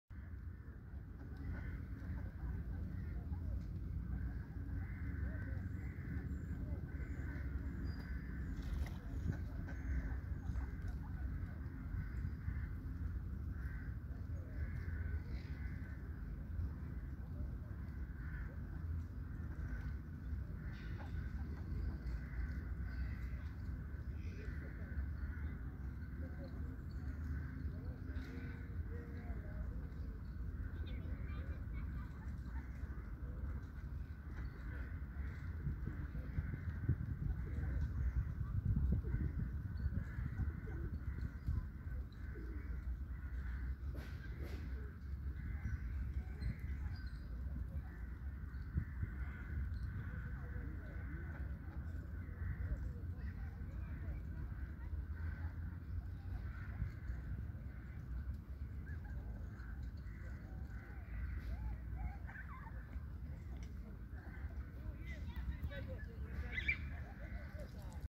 Гомель. Утки на озере в сквере "Фестивальный"
Молодняк становится на крыло. Много уток в гомельском сквере  "Фестивальный"